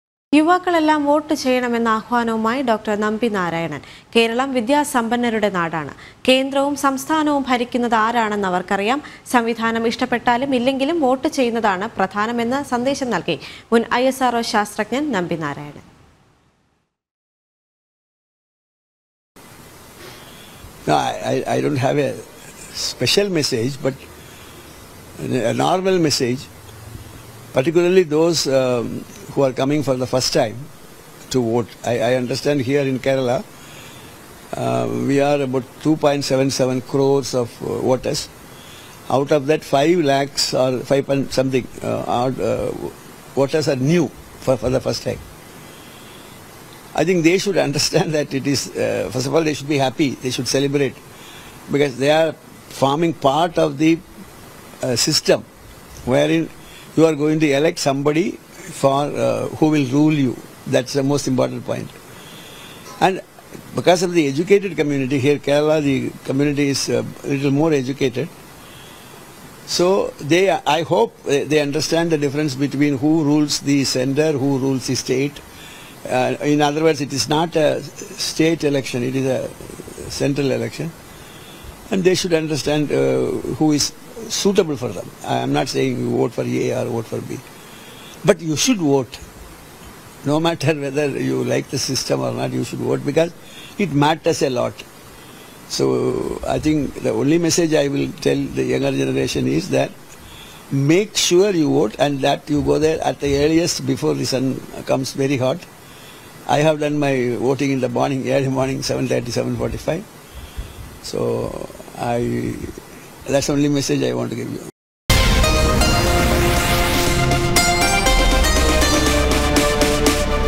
യുവാക്കളെല്ലാം വോട്ട് ചെയ്യണമെന്ന ആഹ്വാനവുമായി ഡോ.നമ്പി നാരായണൻ |LOKSABHA ELECTION
യുവാക്കളെല്ലാം വോട്ട് ചെയ്യണമെന്ന ആഹ്വാനവുമായി ഡോ.നമ്പി നാരായണൻ. കേരളം വിദ്യാസമ്പന്നരുടെ നാടാണ്. കേന്ദ്രവും സംസ്ഥാനവും ഭരിക്കുന്നത് ആരാണ് എന്ന് അവർക്കറിയാം. സംവിധാനം ഇഷ്ടപ്പെട്ടാലും ഇല്ലെങ്കിലും വോട്ട് ചെയ്യുന്നതാണ് പ്രധാനം എന്ന് സന്ദേശം നൽകി മുൻ ISRO ശാസ്ത്രജ്ഞൻ നമ്പി നാരായണൻ.